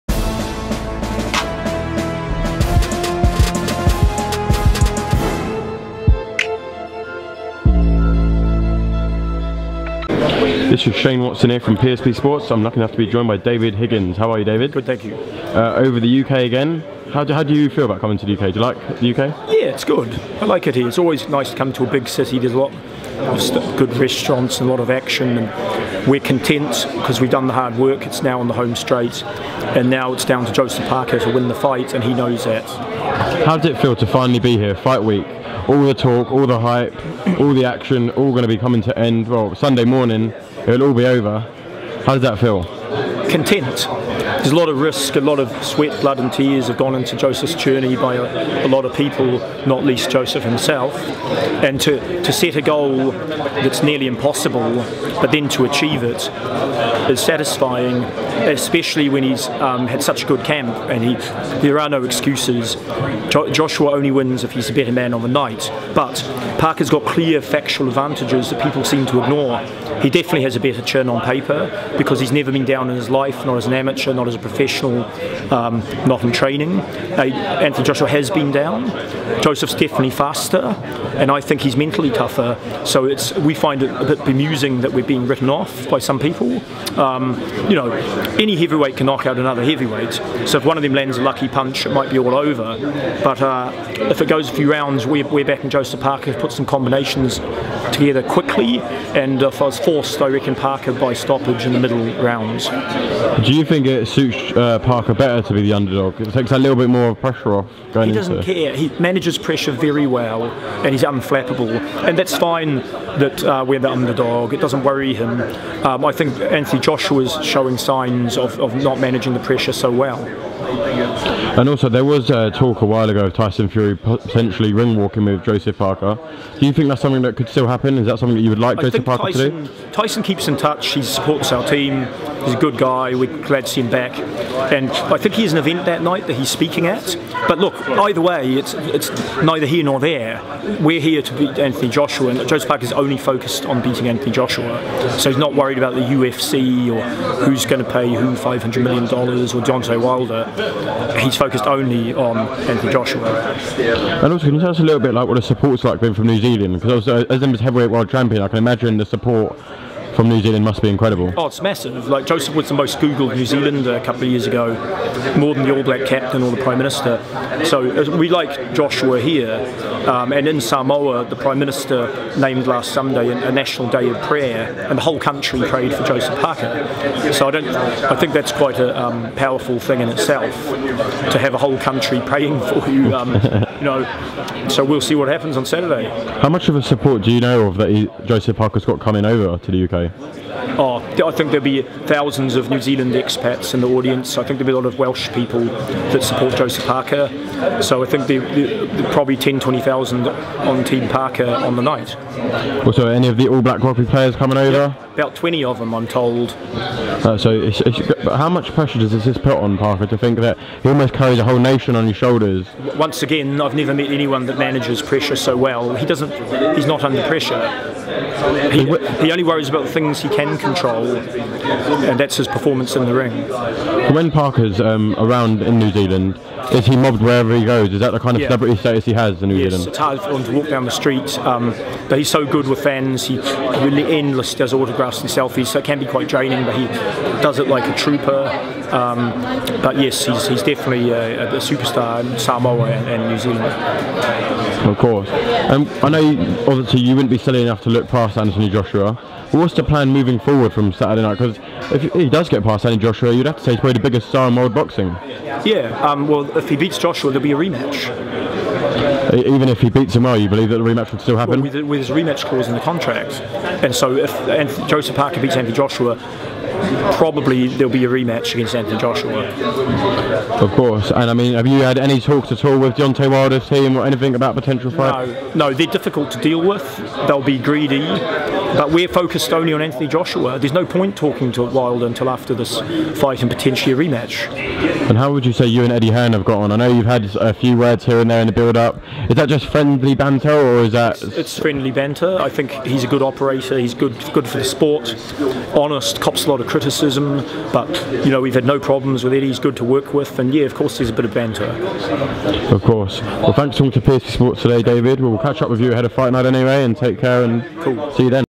DAVID HIGGINS REACTS TO JOSHUA-PARKER PRESS CONFERENCE,ON EDDIE HEARN,PREVIEWS THE FIGHT
INTERVIEW BY SHANE WATSON @shanewatson95 DAVID HIGGINS REACTS TO JOSHUA-PARKER PRESS CONFERENCE,ON EDDIE HEARN,PREVIEWS THE FIGHT